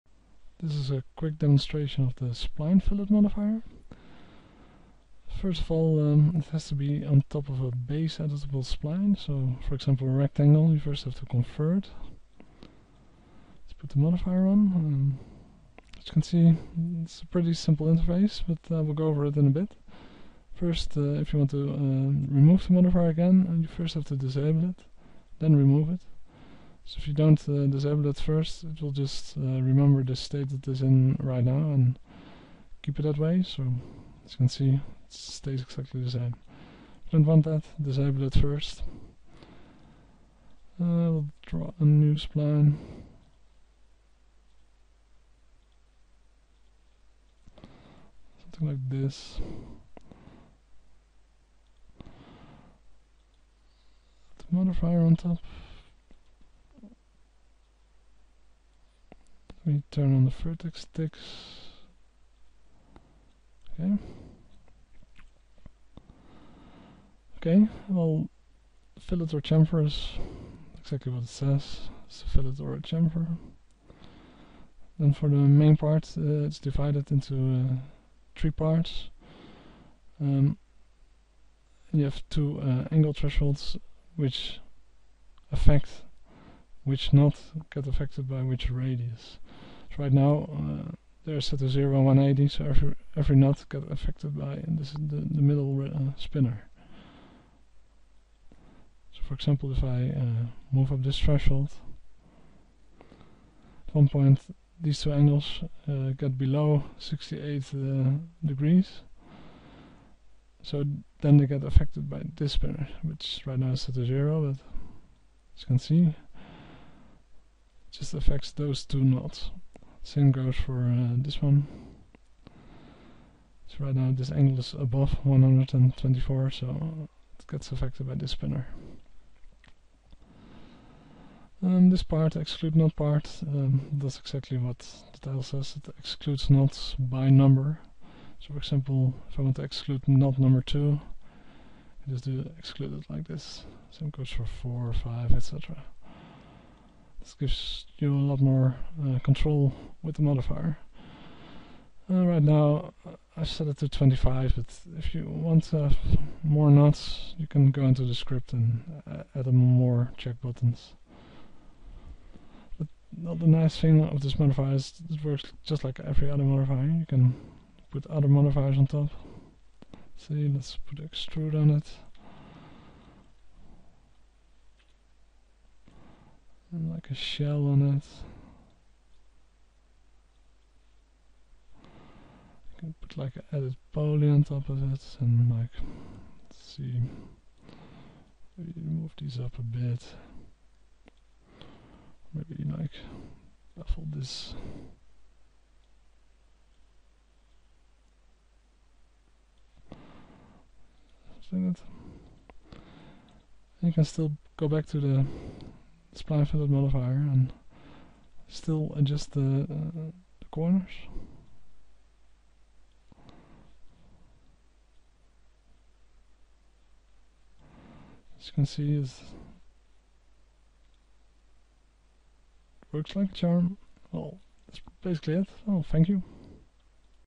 Maxscript: Spline fillet modifier
Modifier for 3ds max that fillets or chamfers knot
corners. Perfect for changing the chamfer or fillet
radius quickly after other modifiers
have been added.